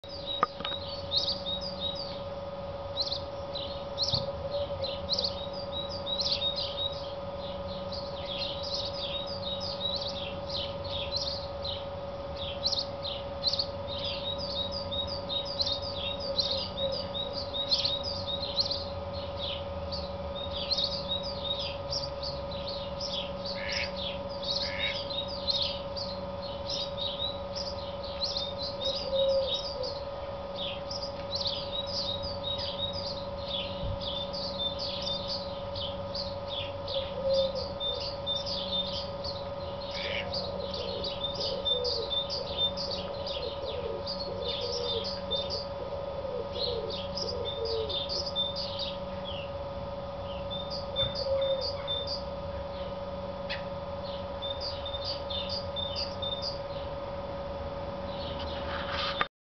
[OLD] male house sparrow advertizing on 03th of april 2008, op het erf van Stichting Witte Mus
dakMusAdverterend03apr2009_5120